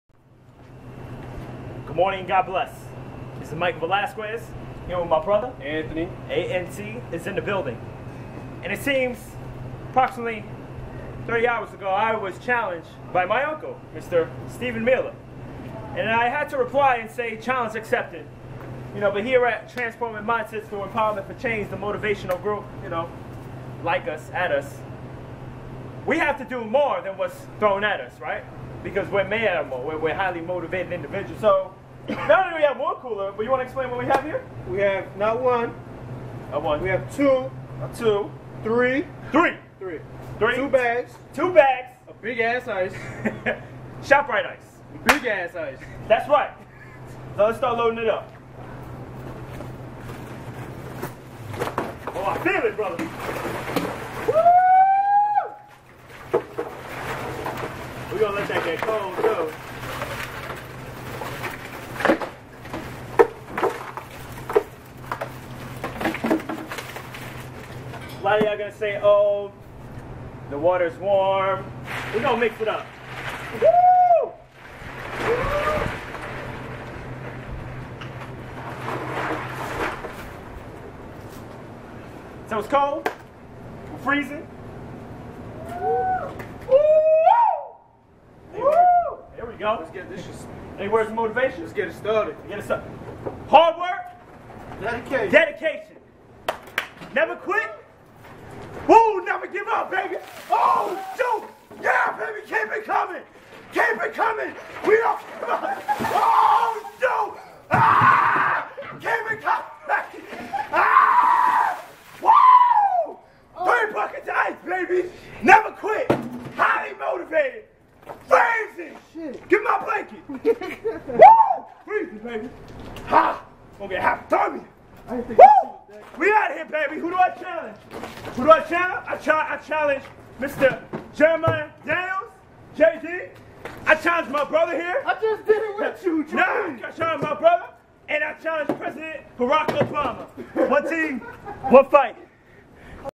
ALS Ice Bucket Challenge Accepted |TMEC
Good Morning and God Bless, My Uncle Steve Challenged me to perform the Ice Bucket Challenge and I not only accepted the challenge but I did more than what the challenge required. Don't allow life's challenges to bring you down: Challenges aren't meant to bring you down, they are meant to Increase your Character so Step Up!! Praying and hoping the best for ALS's Purpose. One Team, One Fight...

Transforming Mindsets through Empowerment for Change